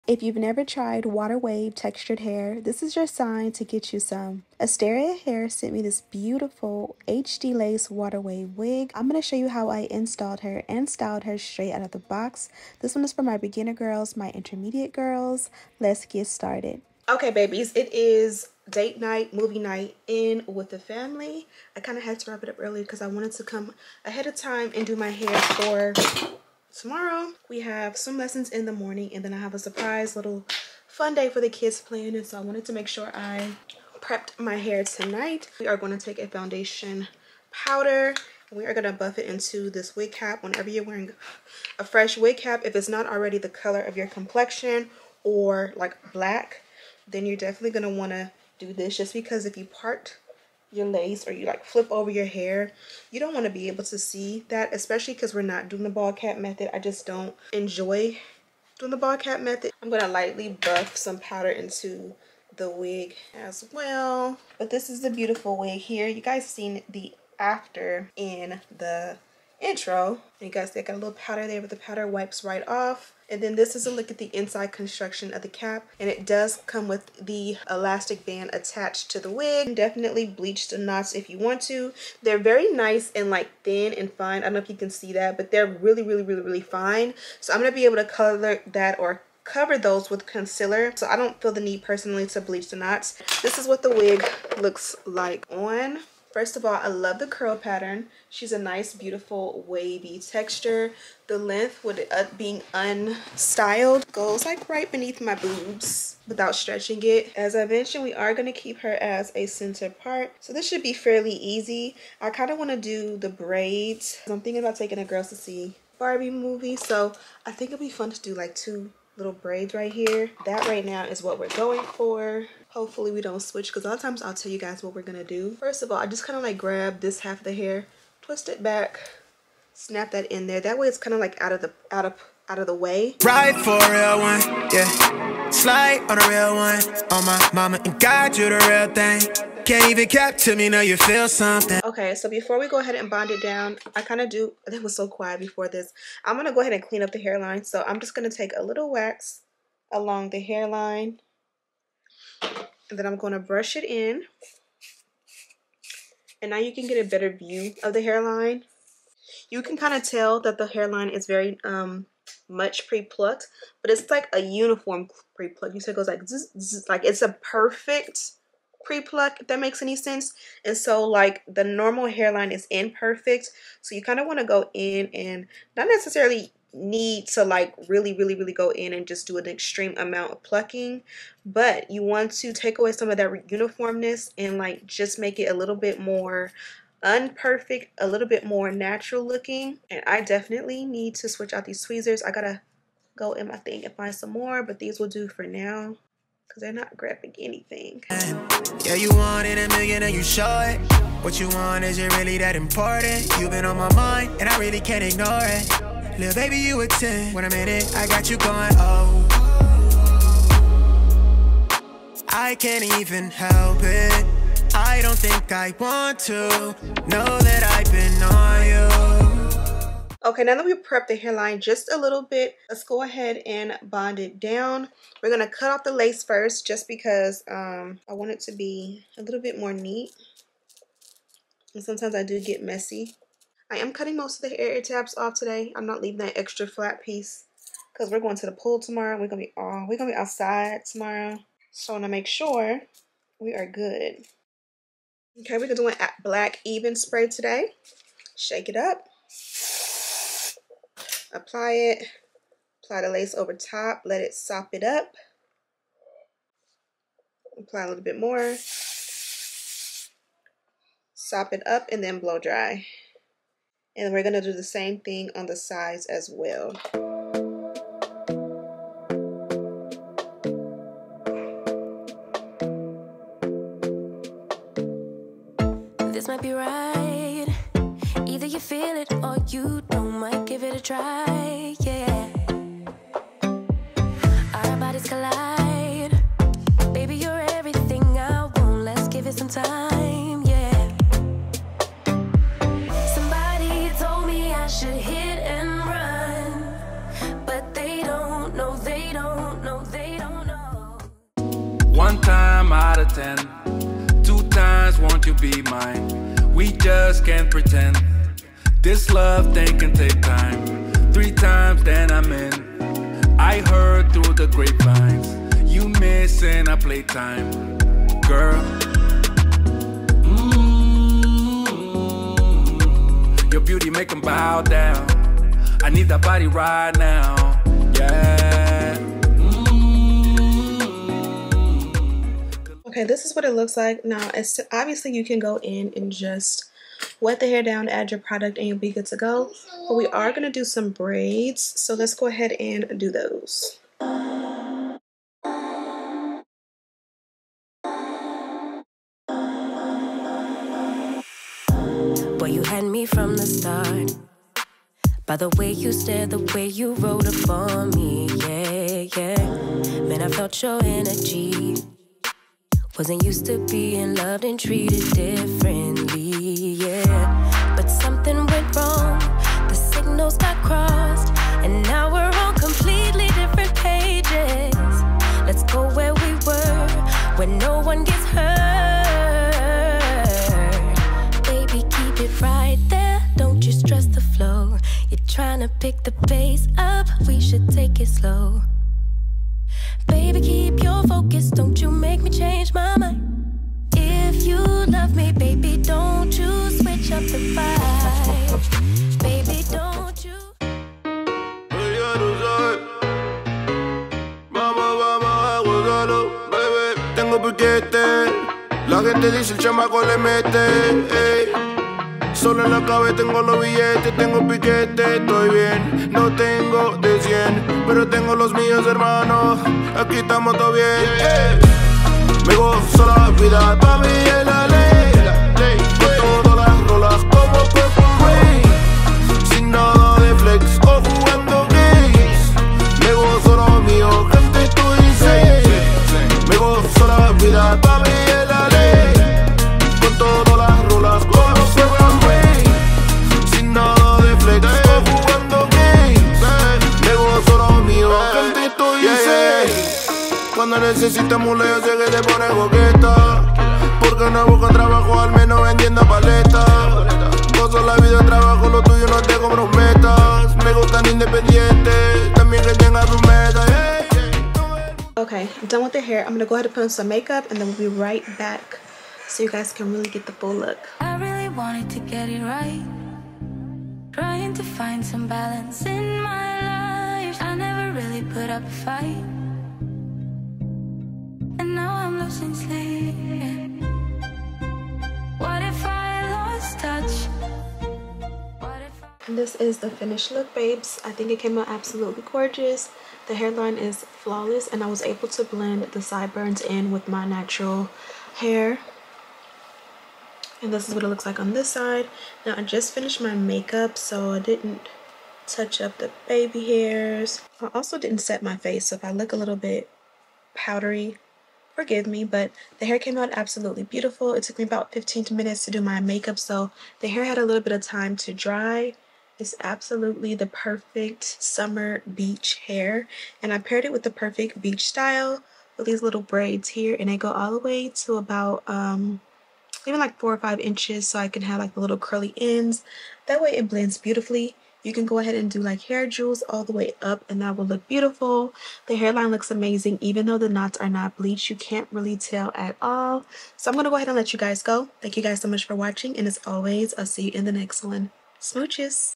PRE PLUCKED WATER WAVE FRONTAL WIG INSTALL + STYLE | PERFECT FOR SUMMER BEACH | ASTERIA HAIR
Hair Info: 24 inch water wave HD 13x4 lace front wig 180% density

▬▬▬ Asteria Hair 7th Anniversary Celebration ▬▬▬
UP TO $85 OFF, CODE: 7year

PRODUCTS MENTIONED:
Eyebrow Razor
Lotta Body Mousse
Cream Foundation (Lace Tint)
Powder Foundation (Lace Tint)

-SMOOCHES 💜💜💜